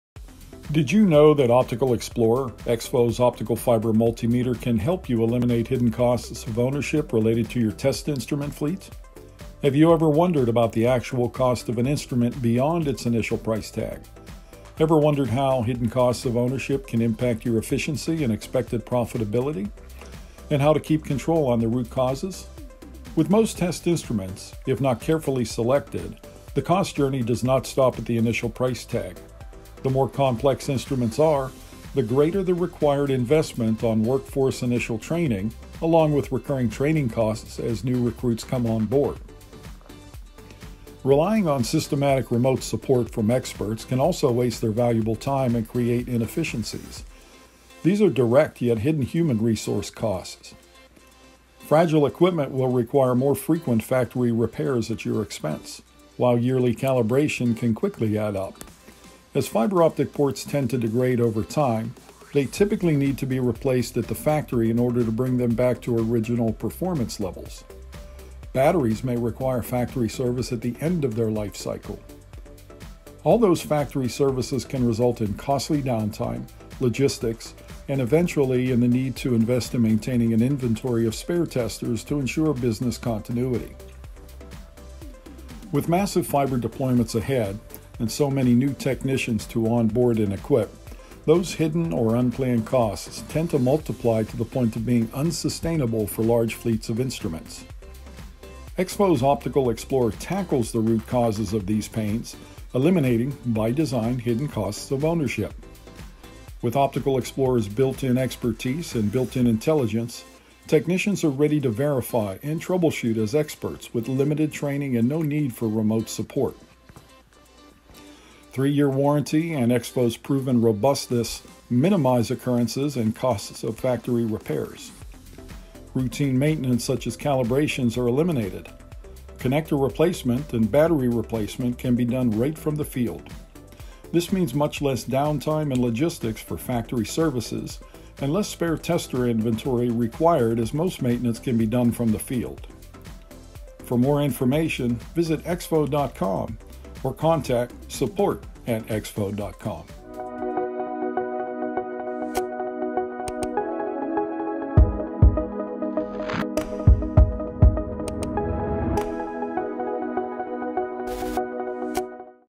How Optical Explorer can help reduce optical tester costs of ownership
Ever wonder about the actual costs of a test instrument beyond its official price tag? Hidden costs of ownership can add up fast, making a big dent in expected profitability. These expenses include yearly in-factory calibration and servicing as well as ongoing workforce training, to name a few. With massive fiber deployments ahead and so many new technicians requiring onboarding, these expenses will become unsustainable. Watch this video to learn more about Optical Explorer, the industry's first optical fiber multimeter. It tackles the root cause of these pains to eliminate, by design, many hidden costs of ownership.